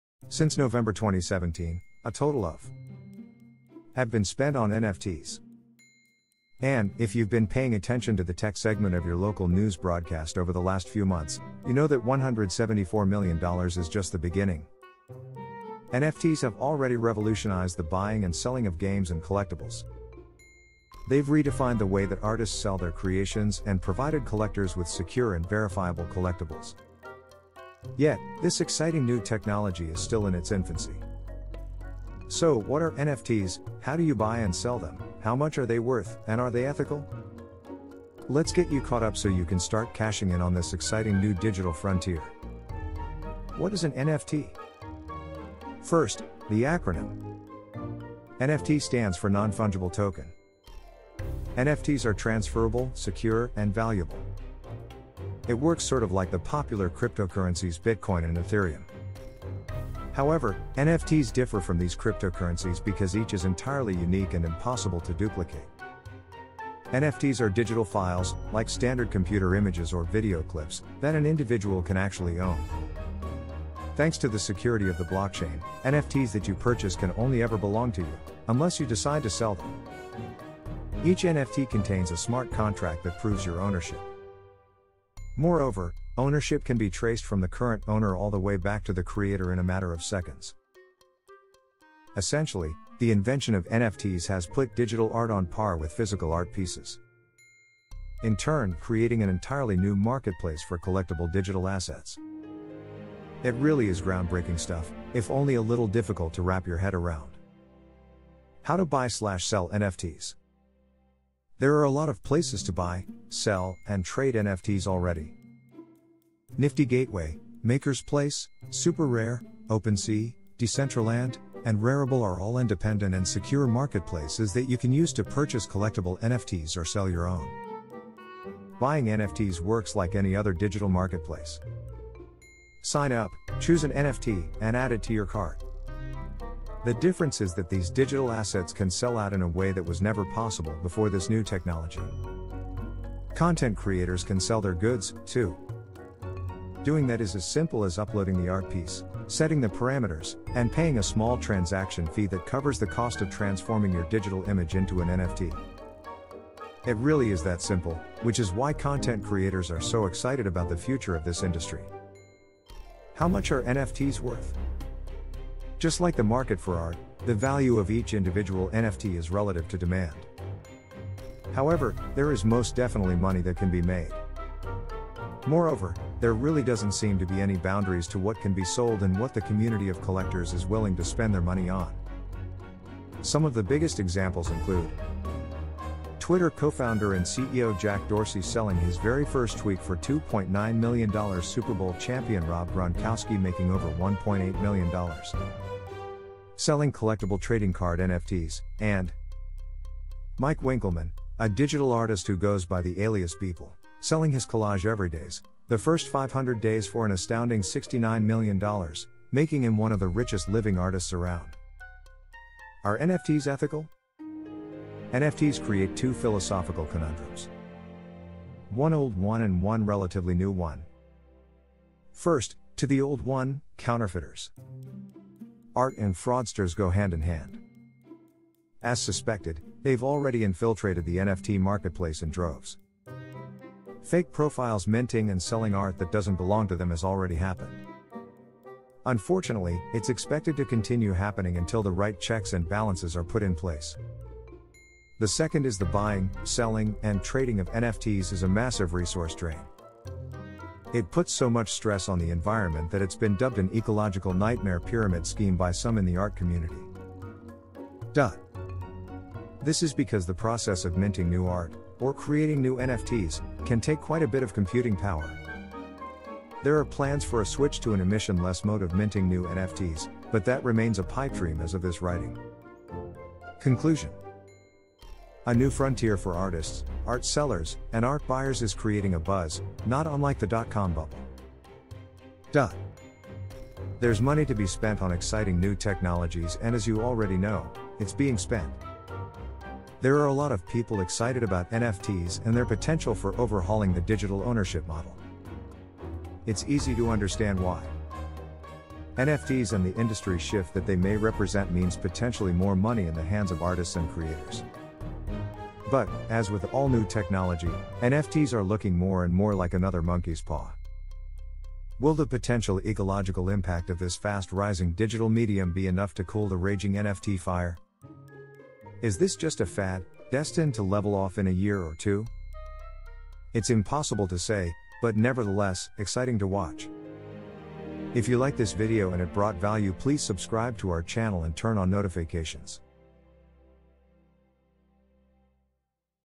What are NFT'S?
Since November 2017, a total of have been spent on NFTs. And, if you’ve been paying attention to the tech segment of your local news broadcast over the last few months, you know that $174 million is just the beginning. NFTs have already revolutionized the buying and selling of games and collectibles. They’ve redefined the way that artists sell their creations and provided collectors with secure and verifiable collectibles. Yet, this exciting new technology is still in its infancy.

So, what are NFTs, how do you buy and sell them, how much are they worth, and are they ethical? Let’s get you caught up so you can start cashing in on this exciting new digital frontier.

What Is an NFT?

First, the acronym. “NFT” stands for “non-fungible token”.

NFTs are transferrable, secure, and valuable. It works sort of like the popular cryptocurrencies Bitcoin and Ethereum. However, NFTs differ from these cryptocurrencies because each is entirely unique and impossible to duplicate.

NFTs are digital files, like standard computer images or video clips, that an individual can actually own. Thanks to the security of the blockchain, NFTs that you purchase can only ever belong to you, unless you decide to sell them. Each NFT contains a smart contract.
that proves your ownership. Moreover, ownership can be traced from the current owner all the way back to the creator in a matter of seconds.
Essentially, the invention of NFTs has put digital art on par with physical art pieces. In turn, creating an entirely new marketplace for collectible digital assets. It really is groundbreaking stuff, if only a little difficult to wrap your head around.

There are a lot of places to buy, sell, and trade NFTs already. Nifty Gateway, MakersPlace, SuperRare, Open Sea, Decentraland, and Rarible are all independent and secure marketplaces that you can use to purchase collectible NFTs or sell your own.

Buying NFTs works like any other digital marketplace. Sign up, choose an NFT, and add it to your cart. The difference is that these digital assets can sell out in a way that was never possible before this new technology.

Content creators can sell their goods, too. Doing that is as simple as uploading the art piece, setting the parameters, and paying a small transaction fee that covers the cost of transforming your digital image into an NFT. It really is that simple, which is why content creators are so excited about the future of this industry.

How Much Are NFTs Worth?

Just like the market for art, the value of each individual NFT is relative to demand. However, there is most definitely money that can be made. Moreover, there really doesn’t seem to be any boundaries to what can be sold and what the community of collectors is willing to spend their money on.

Some of the biggest examples include:
Twitter co-founder and CEO Jack Dorsey selling his very first tweet for $2.9 million.
Super Bowl champion Rob Gronkowski making over $1.8 million
selling collectible trading card NFTs, and;
Mike Winkelmann, a digital artist who goes by the alias Beeple, selling his collage “Everydays: The First 500 Days” for an astounding $69 million, making him one of the richest living artists around.
 It remains to be seen whether the NFT market cools down or heats up. At the moment, it’s evident that NFTs are lucrative.

Are NFTs Ethical?

NFTs create two philosophical conundrums. One old one and one relatively new one.

First, to the old one: counterfeiters. Art and fraudsters go hand-in-hand. As suspected, they’ve already infiltrated the NFT marketplace in droves. Fake profiles minting and selling art that doesn’t belong to them has already happened. Unfortunately, it’s expected to continue happening until the right checks and balances are put in place.

The second is the buying, selling, and trading of NFTs is a massive resource drain. It puts so much stress on the environment that it’s been dubbed an “ecological nightmare pyramid scheme” by some in the art community.

. This is because the process of “minting” new art, or creating new NFTs, can take quite a bit of computing power.

There are plans for a switch to an emission-less mode of minting new NFTs, but that remains a pipedream as of this writing.
Conclusion

A new frontier for artists, art sellers, a and art buyers is creating a buzz, not unlike the

. There’s money to be spent on exciting new technologies and as you already know, it’s being spent.

There are a lot of people excited about NFTs and their potential for overhauling the digital ownership model. It’s easy to understand why. NFTs and the industry shift that they may represent means potentially more money in the hands of artists and creators. But, as with all new technology, NFTs are looking more and more like another monkey’s paw.

 cardano btc NFT NFTS NFT's explained NFT 101